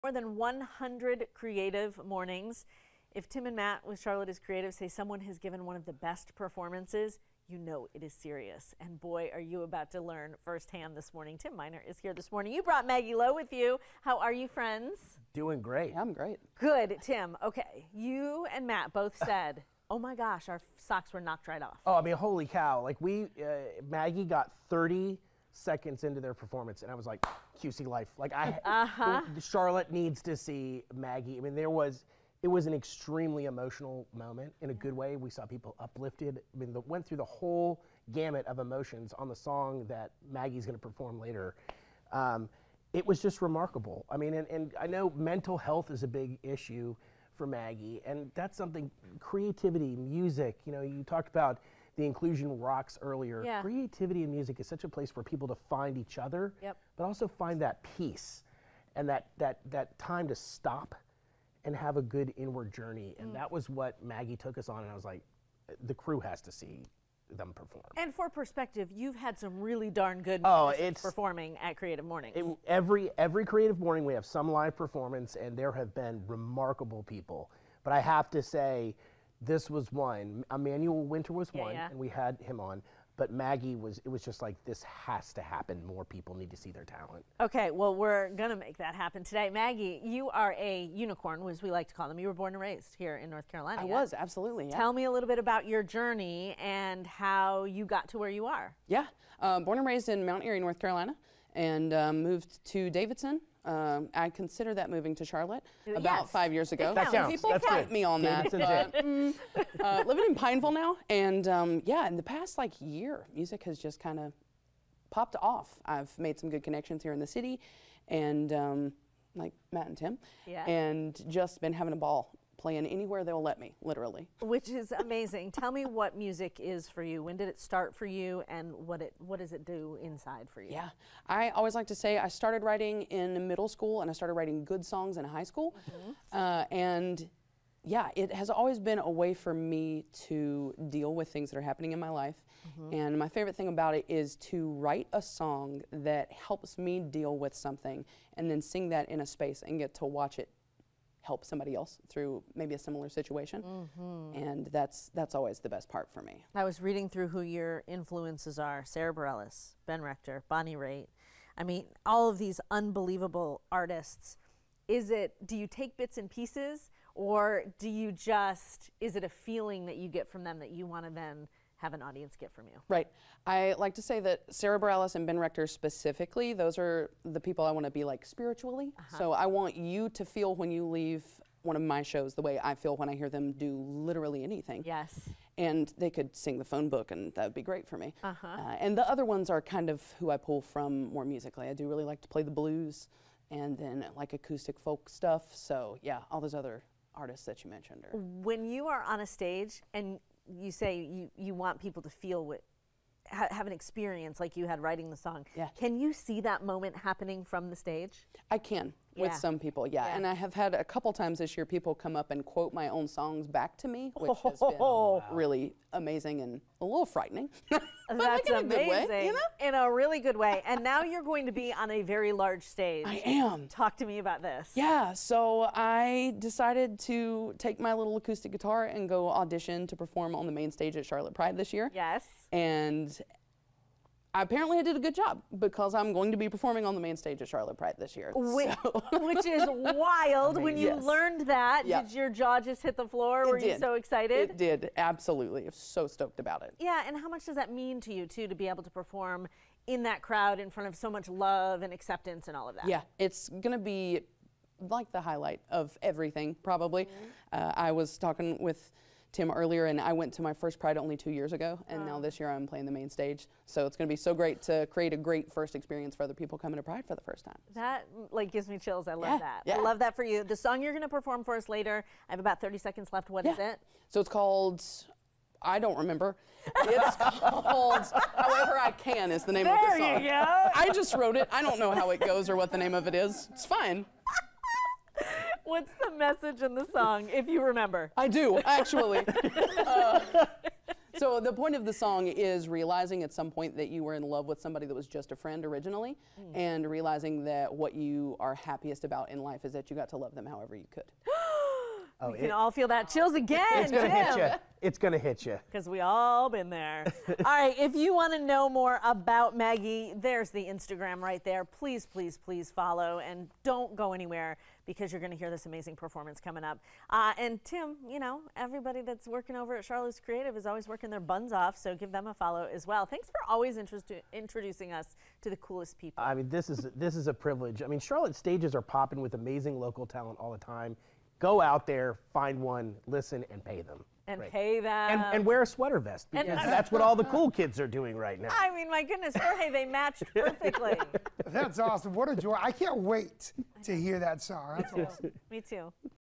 Local artist ready for the main stage
Local musician Maggie Lowe to perform on the main stage at Charlotte Pride